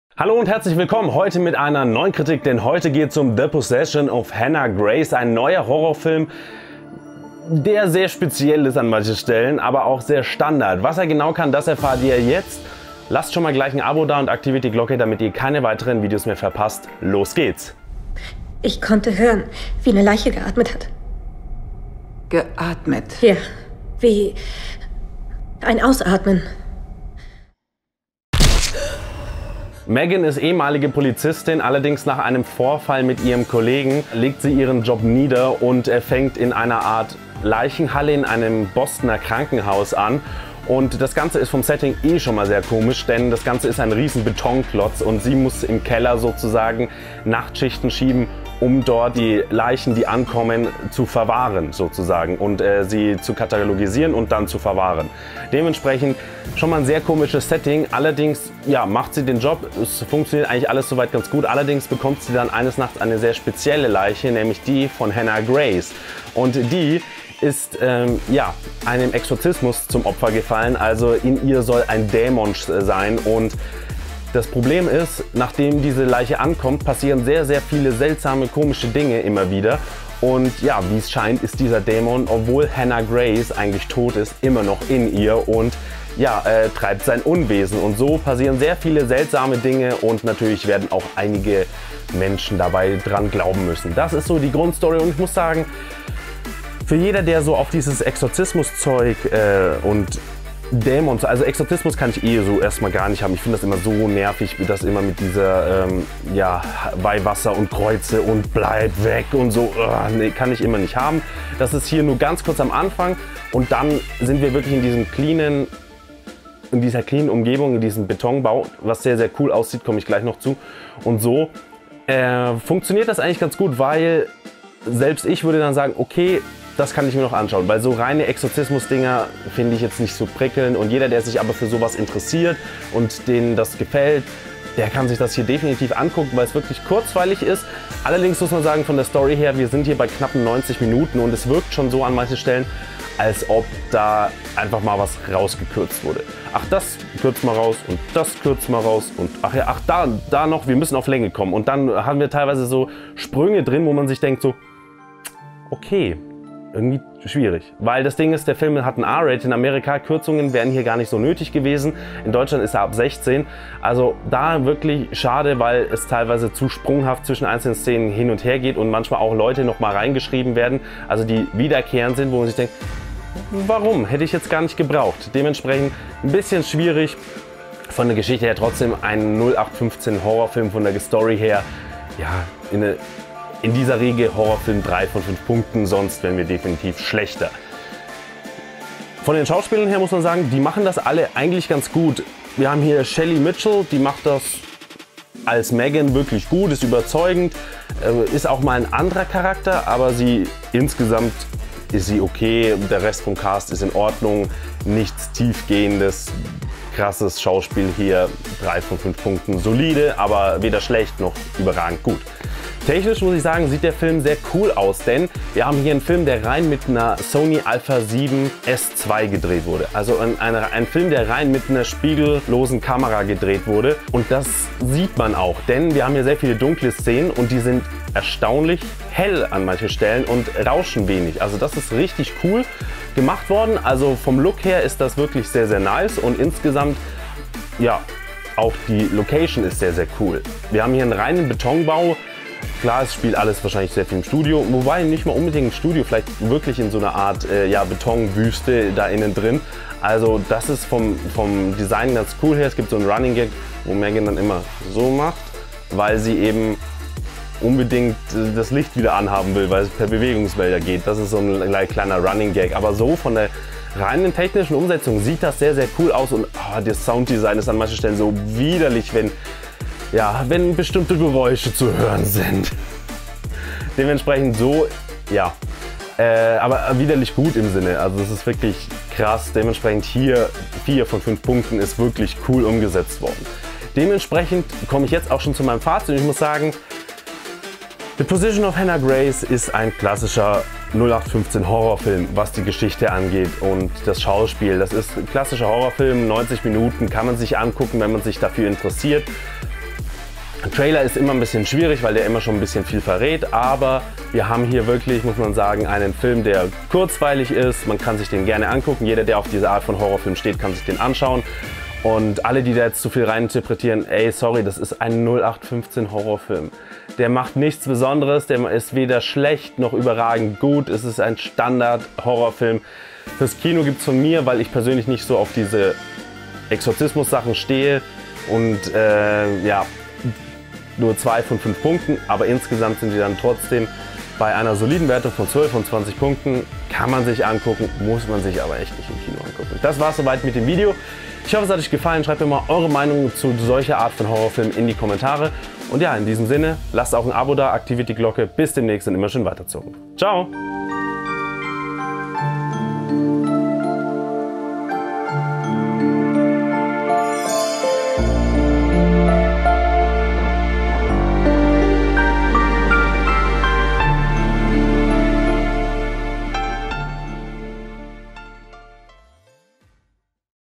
THE POSSESSION OF HANNAH GRACE / Kritik - Review | MYD FILM
Hier meine Kritik zu THE POSSESSION OF HANNAH GRACE.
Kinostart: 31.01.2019
Länge: 85 min.
FSK 16

Für jeden, über ein Sternchen markierten, Link bekomme ich eine kleine Provision vom Anbieter. Ihr bezahlt keinen Cent mehr und könnt mich dadurch unterstützen.